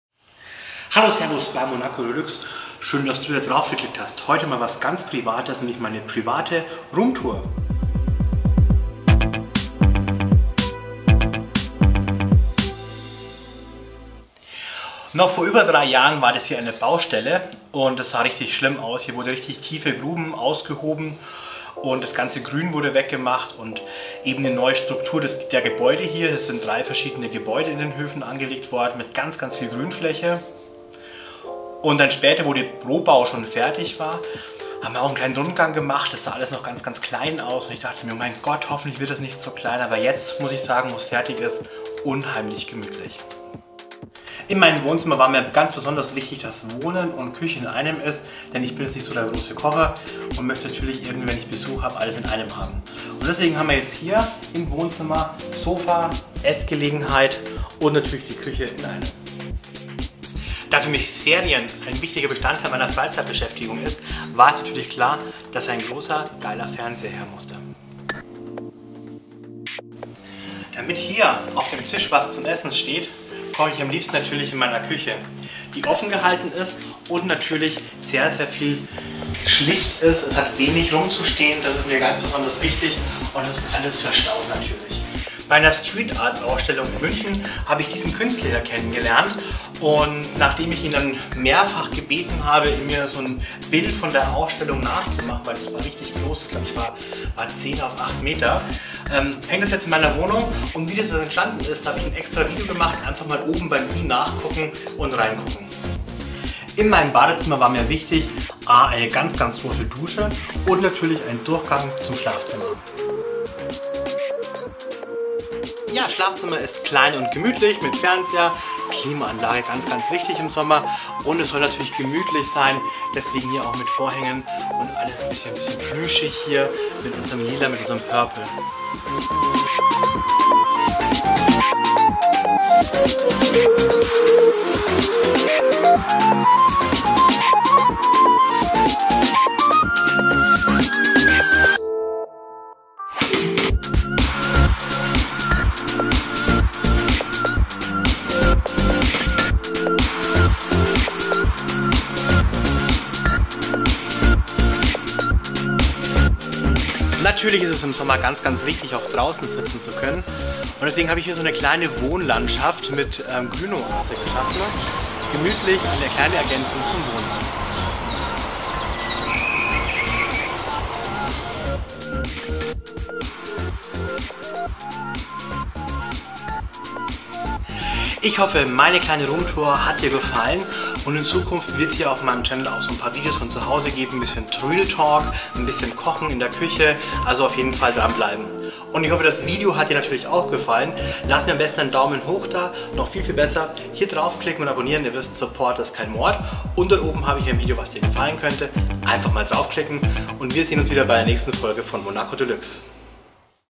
Roomtour, meine neue Wohnung in Regensburg
Endlich meine erste Roomtour! Hoffe das Video gefällt euch!

►Sonstige Ausrüstung?